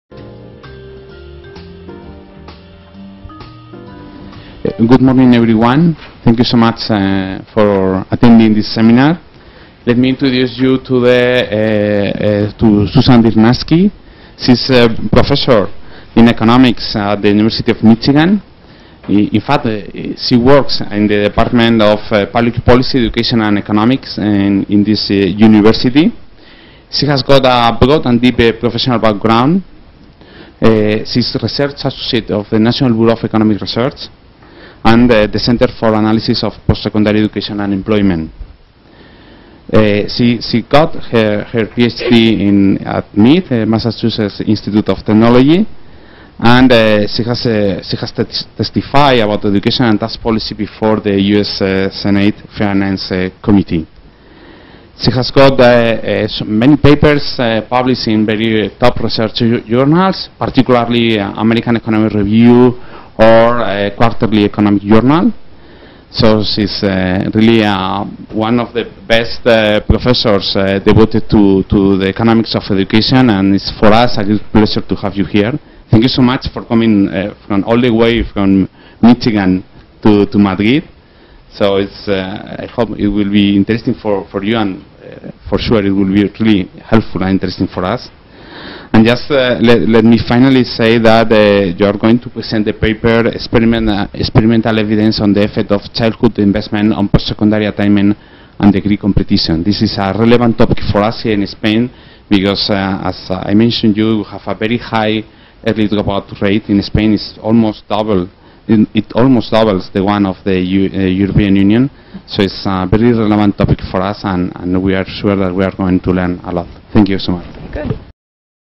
INEE MECD.Susan Dynarski (1): el impacto a largo plazo del tamaño del aula en Educación primaria
¿Ir a clases más reducidas en Educación Primaria favorece el rendimiento de los alumnos? ¿Qué otros outcomes educativos y sociales se encuentran? En estas dos preguntas Susan Dynarski, profesora adjunta en el National Bureau of Economic Research, centra el análisis de esta ponencia.